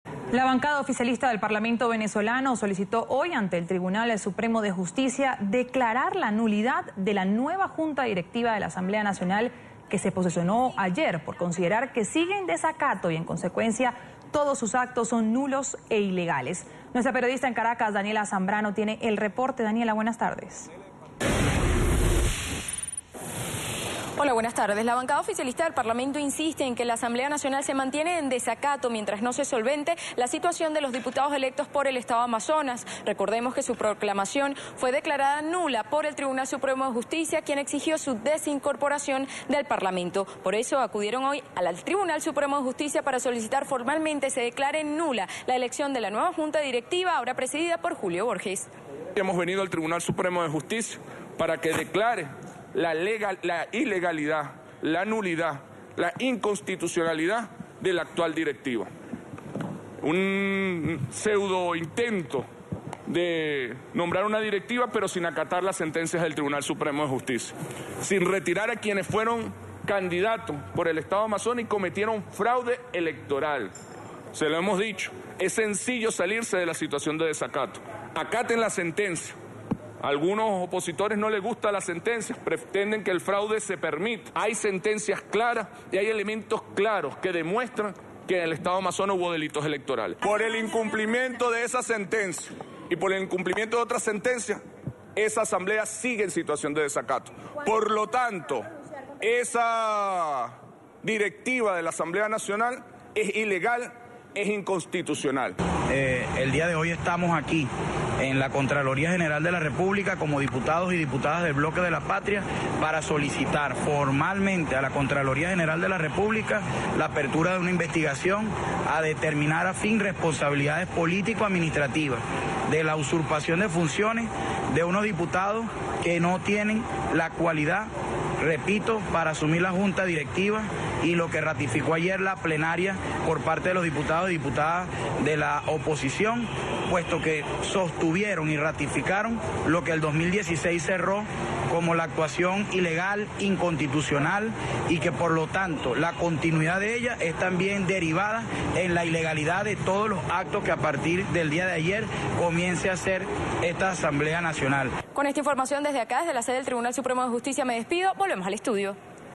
PSUV pide al TSJ declarar la nulidad de la instalación de la nueva directiva parlamentaria
El jefe de la bancada del Polo Patriótico, diputado Héctor Rodríguez, acudió este viernes al Tribunal Supremo de Justicia y solicitó declarar nula la actual junta directiva de la Asamblea Nacional de Venezuela juramentada este jueves 05 de enero.